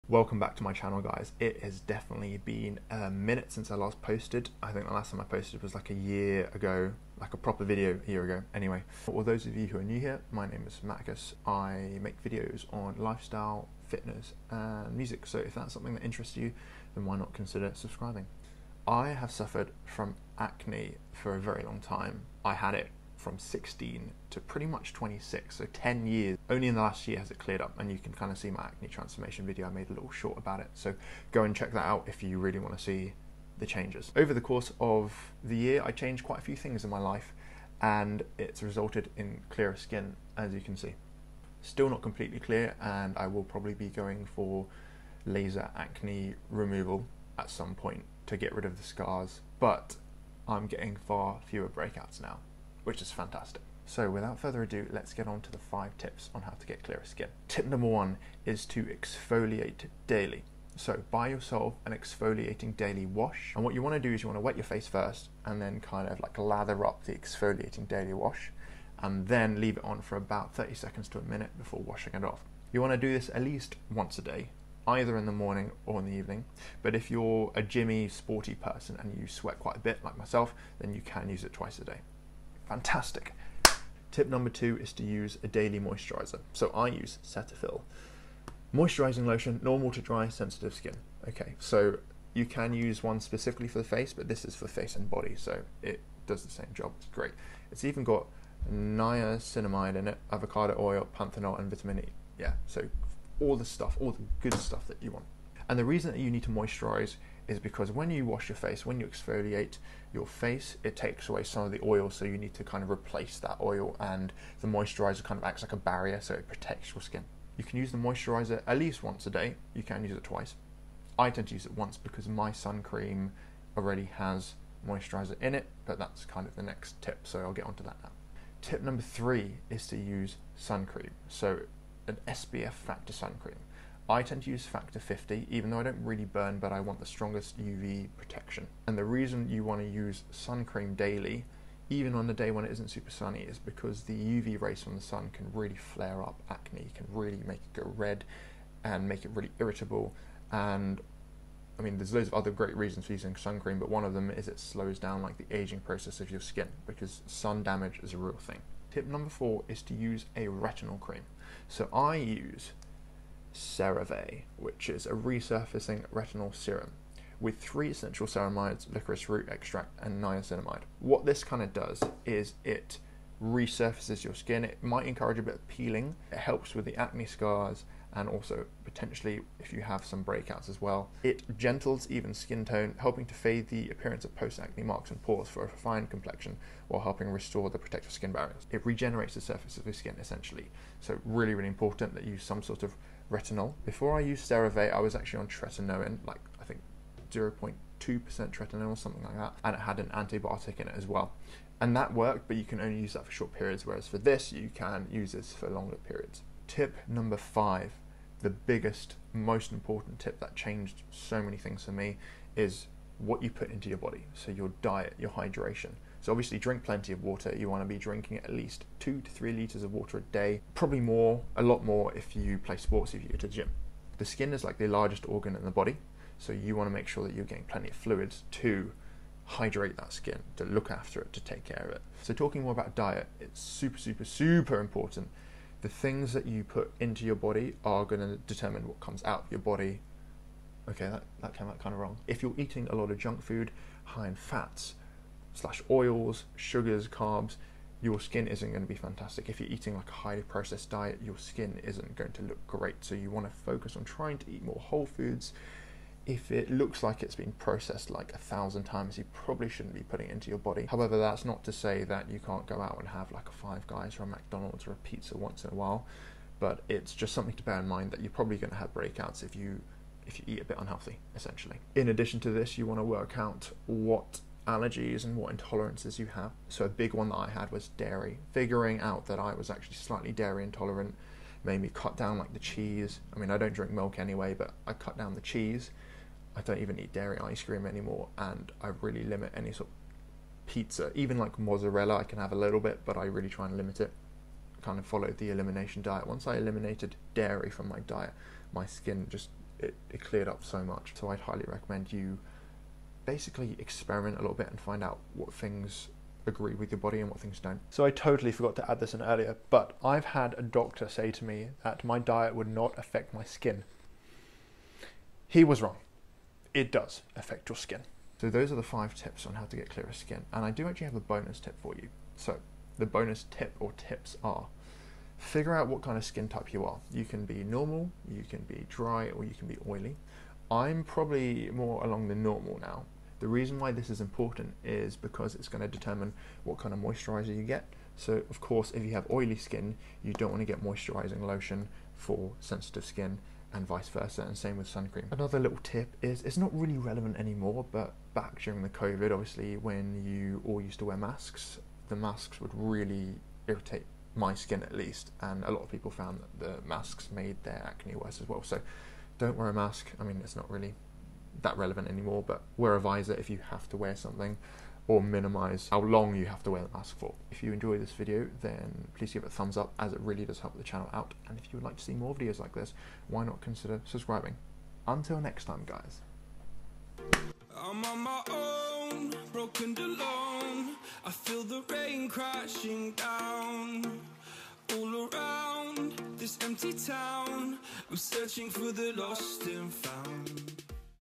How I Achieved Clearer Skin in 5 Easy Steps
5 Tips For Clearer Skin | My Skincare Routine | MATTICUS

Hey all,

So I thought I'd make a video on my skincare routine, and how to get clearer skin.

I was thinking a how to groom your eyebrows or something like that? Haha

Timestamps

0:19 - My Acne Struggles
0:57 - Tip 1
1:26 - Tip 2
2:09 - Tip 3
2:39 - Tip 4
3:27 - Tip 5

4:33 - Allergies and Intolerances

My weekly fitness template:

Aesthetic body workout program: